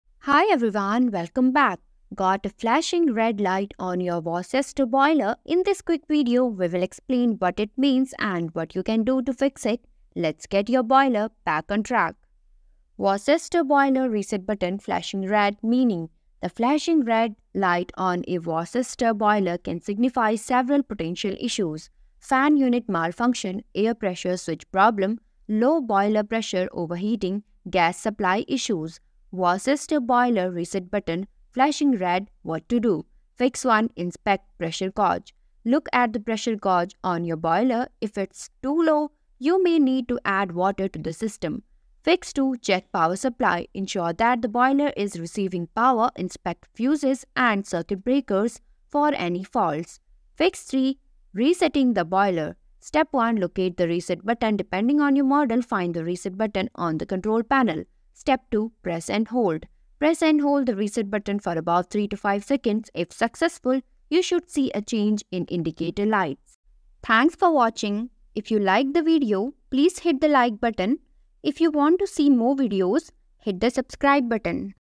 Worcester Boiler Reset Button Flashing Red: Meaning & What To Do?
In this informative video, we delve into the significance of a flashing red reset button on Worcester boilers. Viewers will learn about the potential causes of this warning signal and the appropriate steps to take in order to troubleshoot and resolve the issue effectively. Whether you are a homeowner or a professional, this guide will provide valuable insights into maintaining your boiler's performance.

00:00 Introduction
00:12 Worcester Boiler Reset Button Flashing Red: Meaning
00:29 Worcester Boiler Reset Button Flashing Red: What To Do?